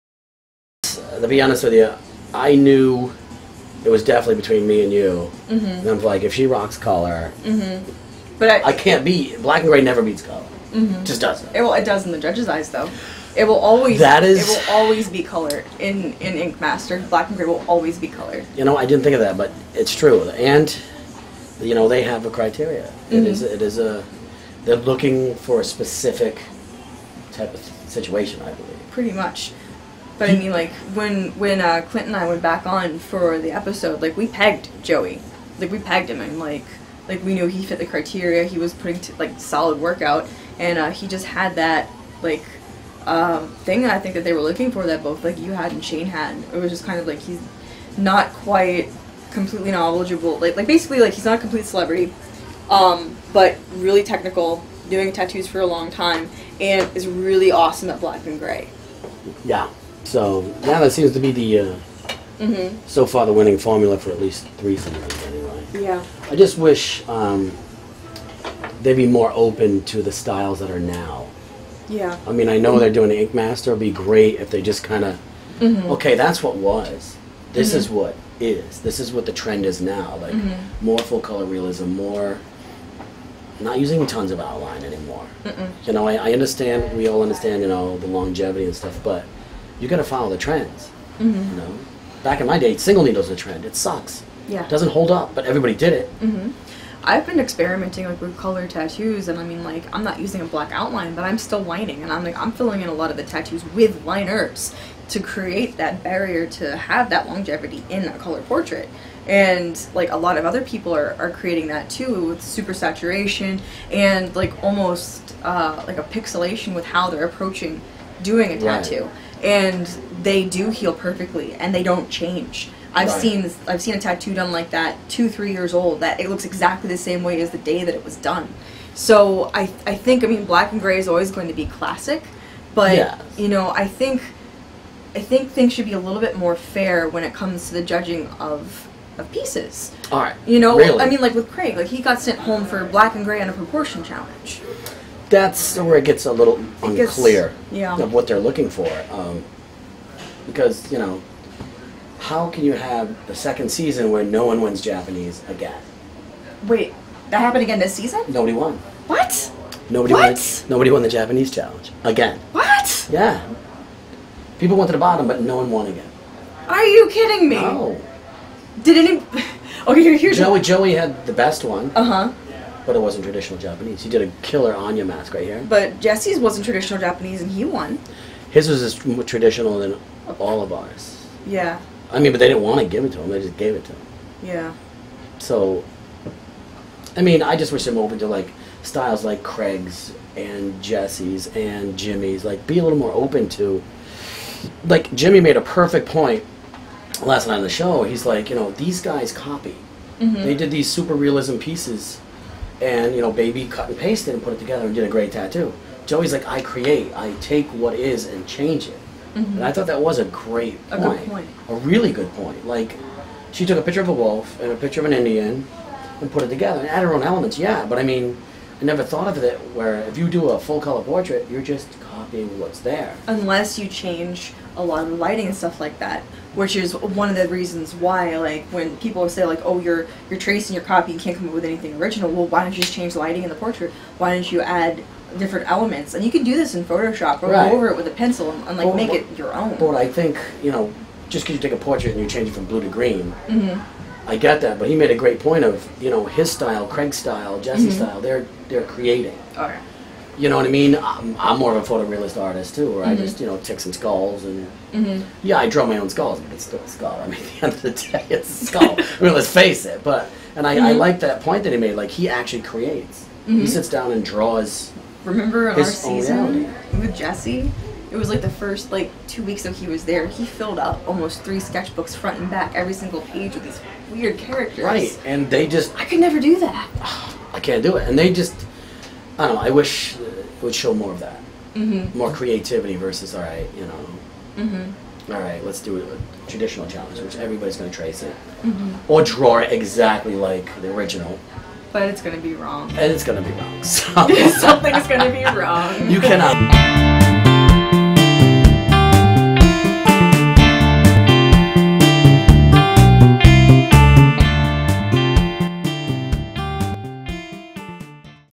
Steve Tefft & Sarah Miller Talk "Ink Master" With Rebel Ink Magazine
"Ink Master" Season 2 Winner, Steve Tefft and runner up, Sarah Miller talk about their experience as contestants in the Spike TV Tattoo Competition program.